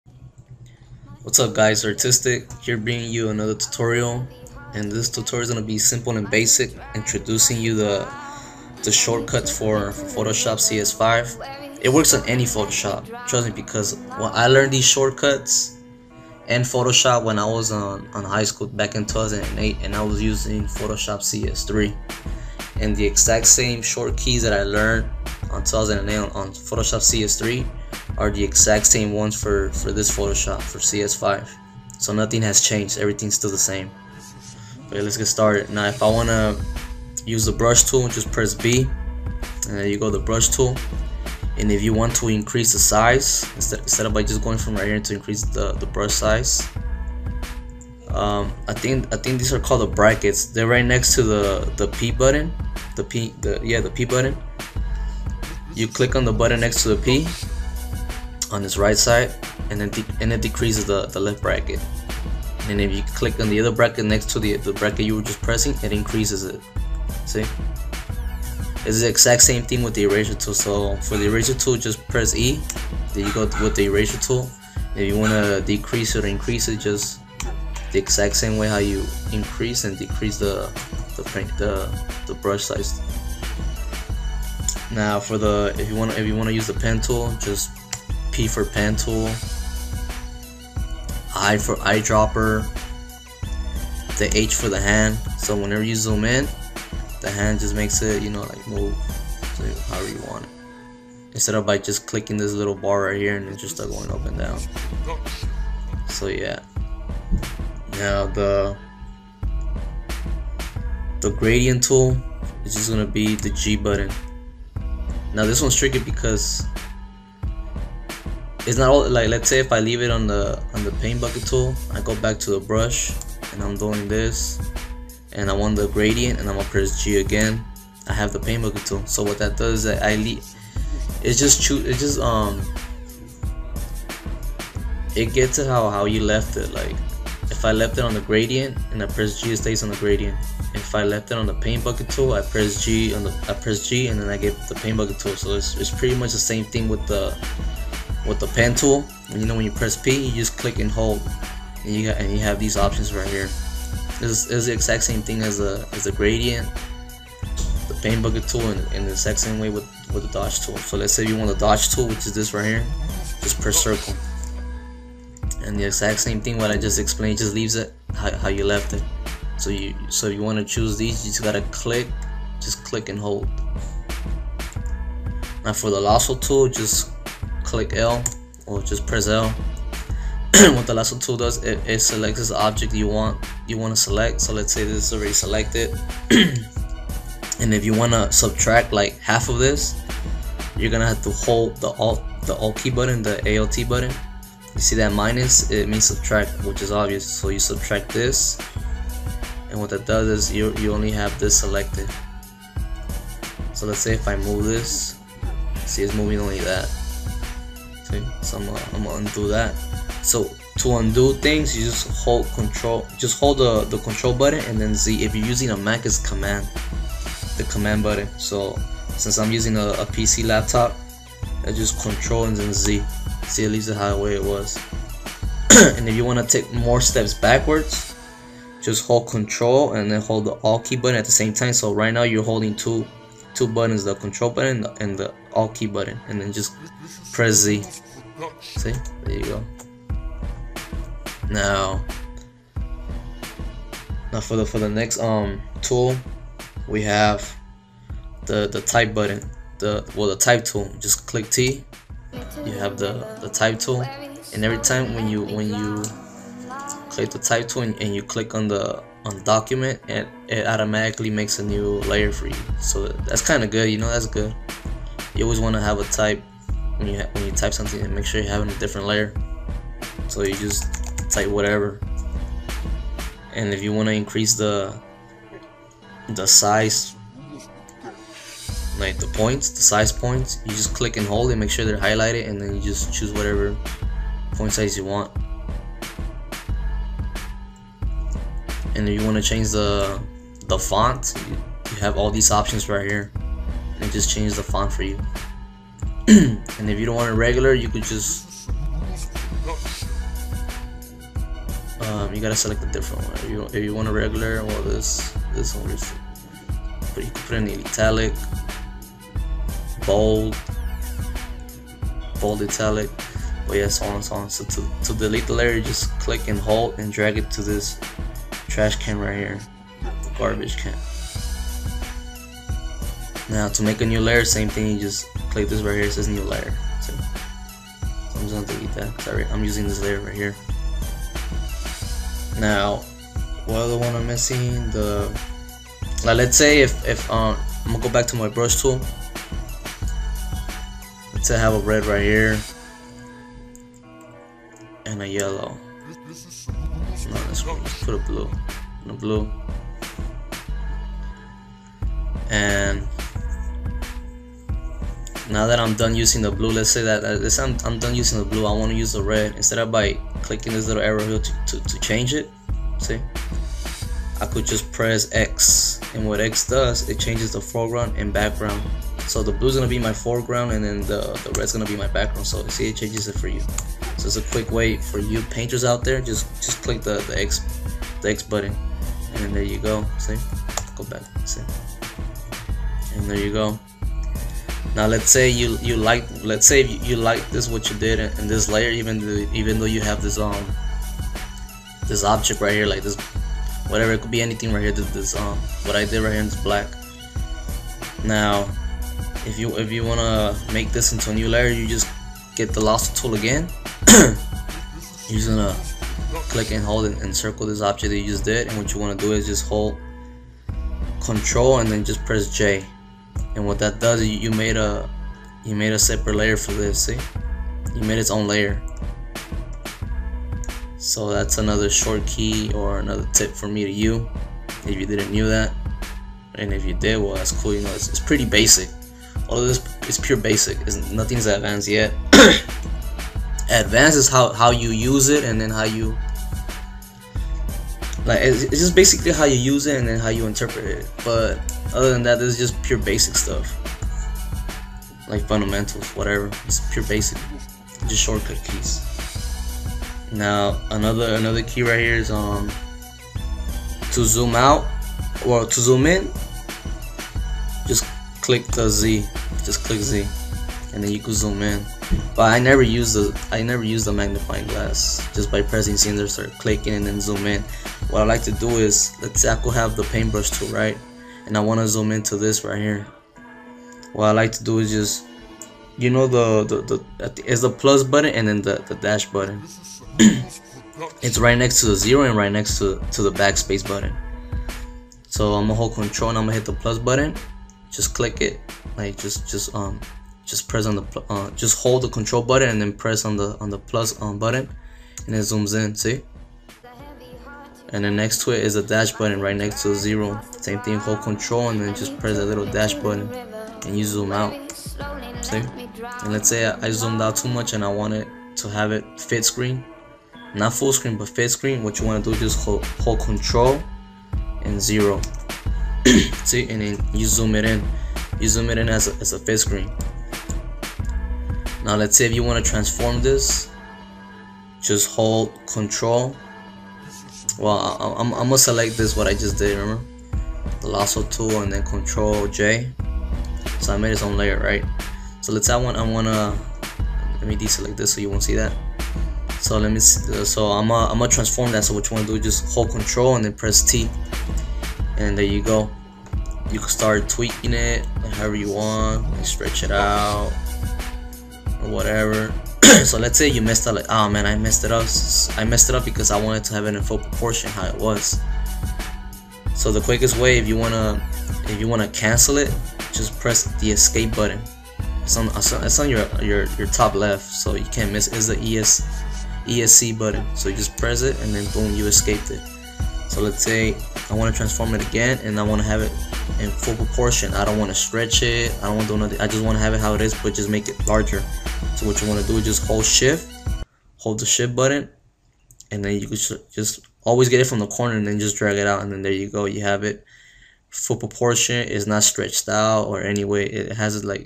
Photoshop CS5 Shortcut Keys Part 1
Shortcut Key List:
B - Brush
[ - Decrease size
] - Increase size
E - Eraser
P - pen
I - Eyedrop
H - Hand tool
G - Gradient
T - Type tool
D - Default tool
O - Dodge/Burn/Sponge tool
M - Rectangular tool
X - Toggle foreground and background
V - Move tool
L - Lasso tool
Hold Shift to increase the selection
Hold Alt to Decrease selection
Z - Zoom
Ctrl + 0 - Fit Screen
Ctrl + = - Zoom in
Ctrl + - - Zoom out
Ctrl + E - Merge down layer
Ctrl + T - Transform
Hold Shift for full proportion
Ctrl + U - Hue/Saturation
Ctrl + A - Select all
Ctrl + D - Deselect
Ctrl + V - Paste
Ctrl + C - Copy
Ctrl + Z - Undo/Redo
Ctrl + Alt + Z - step backwards
Ctrl + J - Make its own layer
Ctrl + N - New Document
Ctrl + Q - Quit